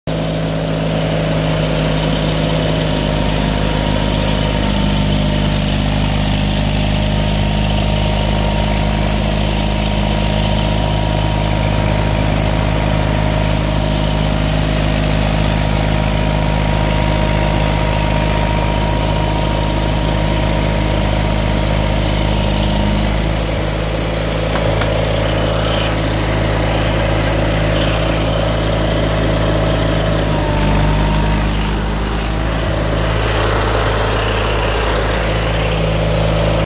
John Deere 318 with Deere 49 snowblower homemade chute
Same tractor and blower as other video. First time using the chute.